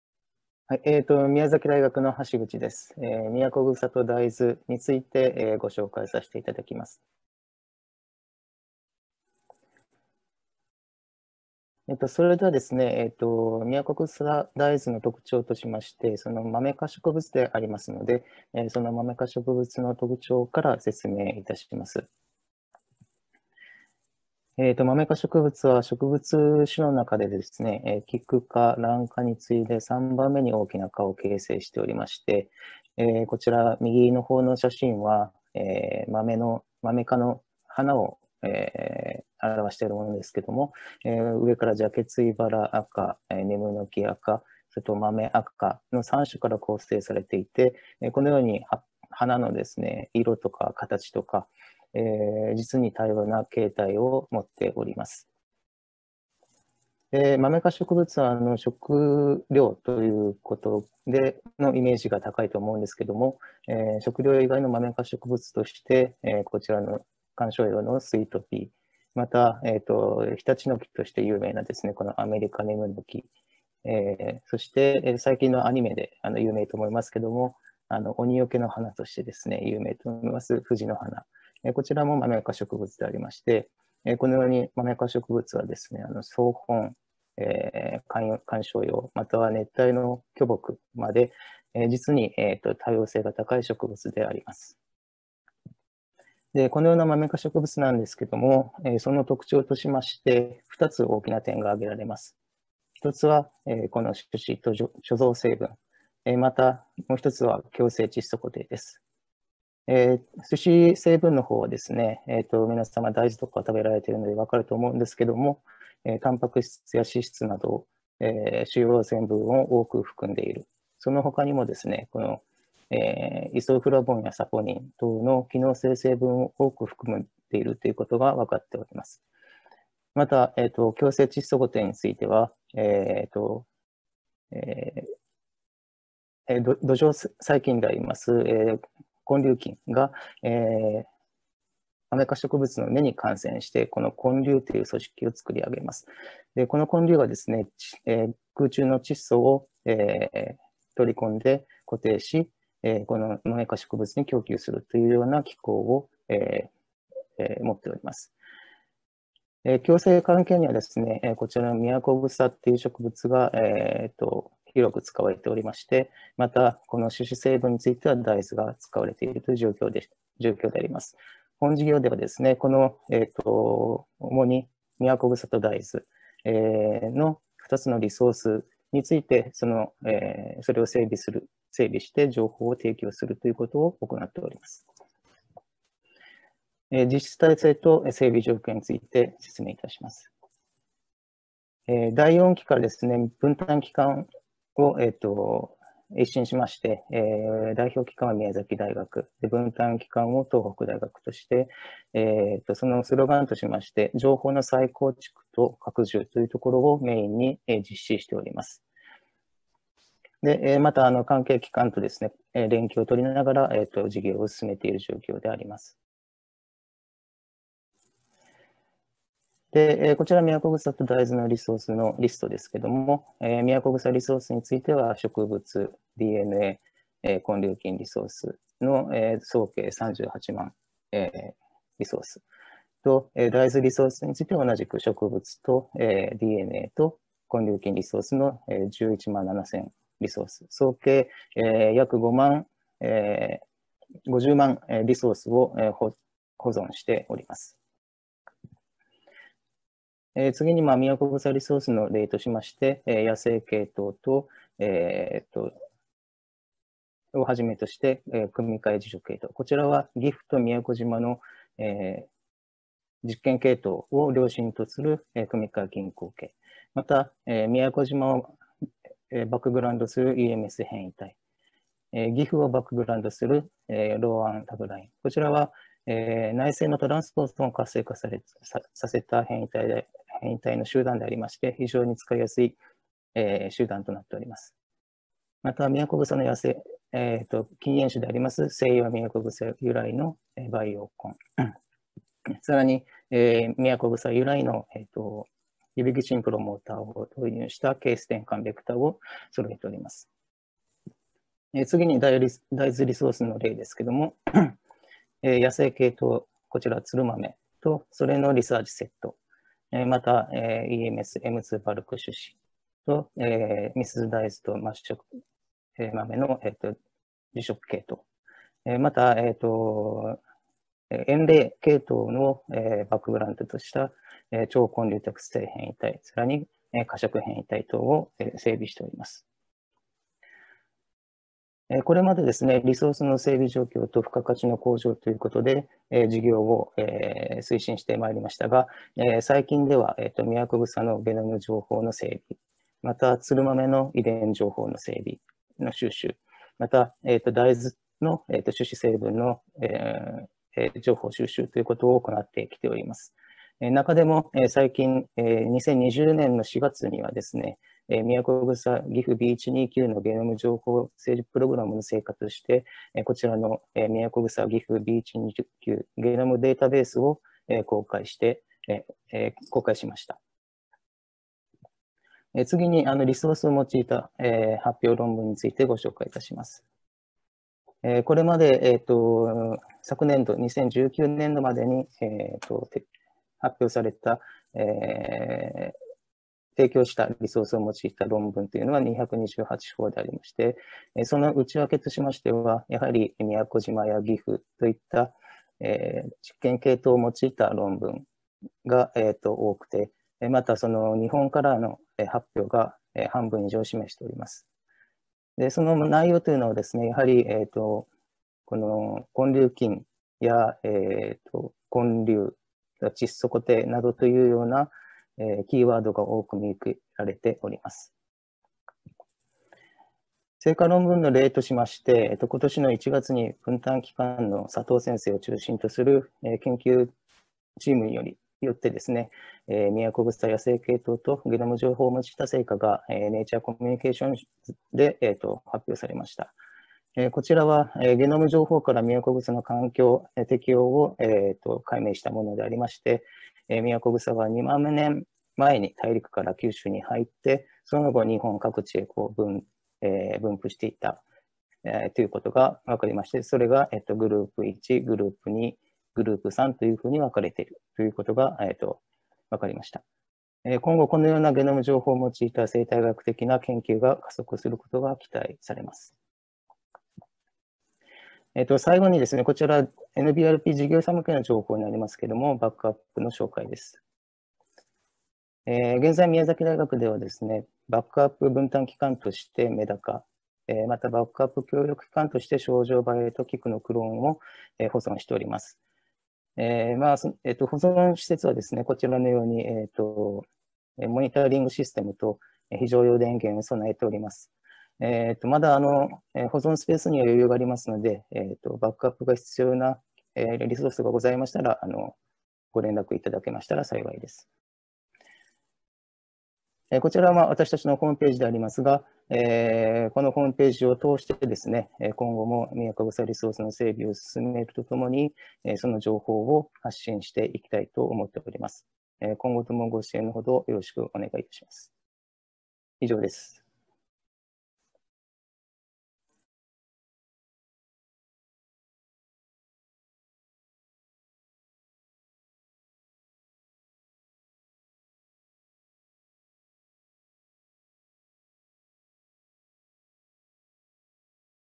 NBRPミヤコグサ・ダイズの紹介（NBRPオンラインフォーラム2020）
講演タイトル：NBRPミヤコグサ・ダイズ　ｰマメ科植物研究のためのバイオリソースｰ
講演者：橋口 正嗣（宮崎大学 農学部）